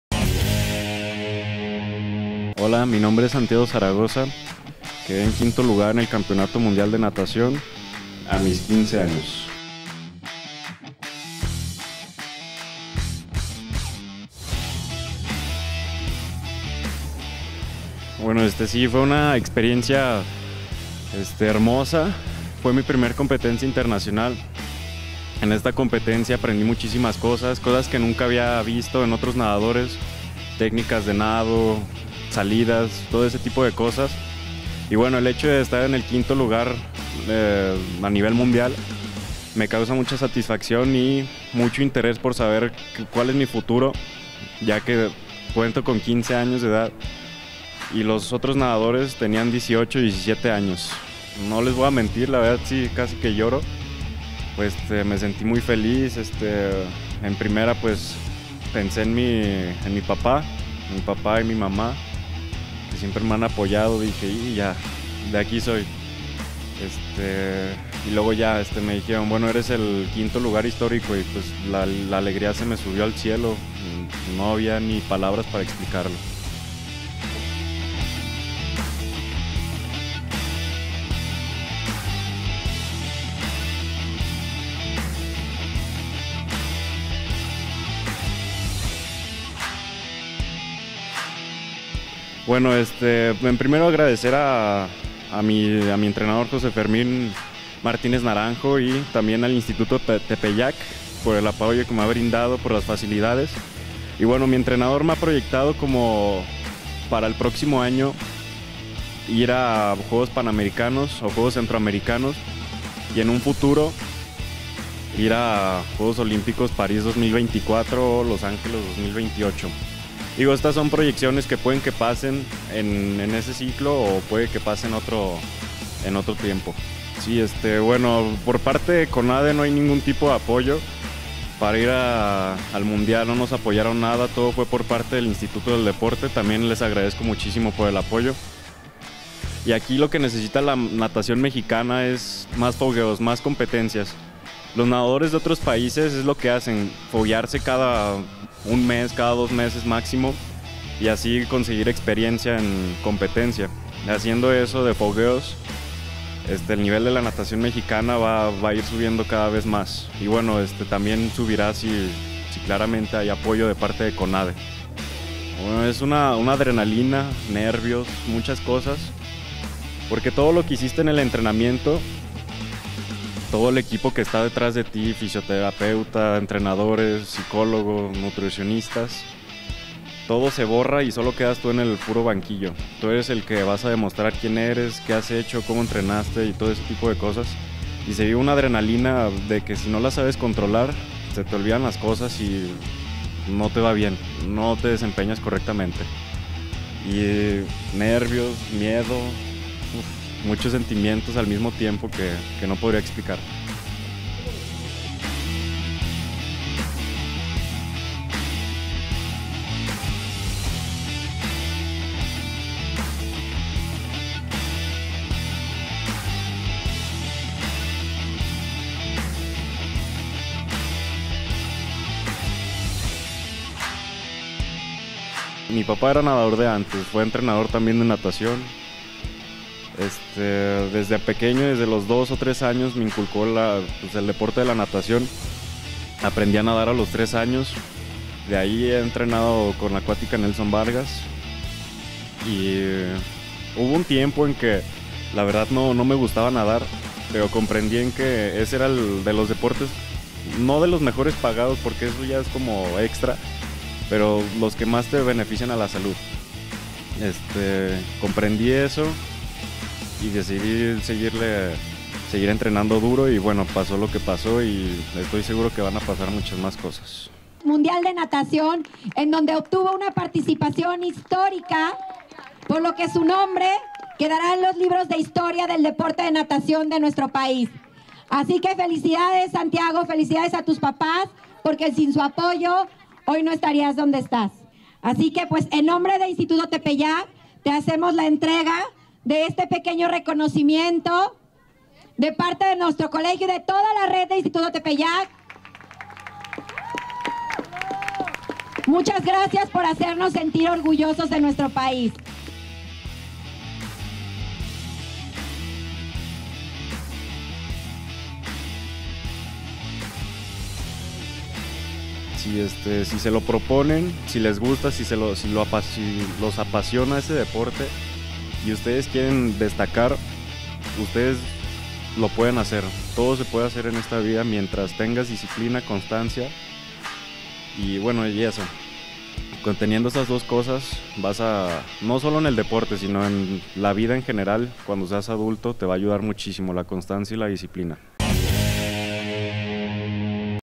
Deportes Zaragoza
Entrevista con Santiago Zaragoza atleta en la disciplina de natación quién recientemente obtuvo el 5° Lugar en el Word Junior Swimming Championships 2022 organizado por la FINA en Lima, Perú.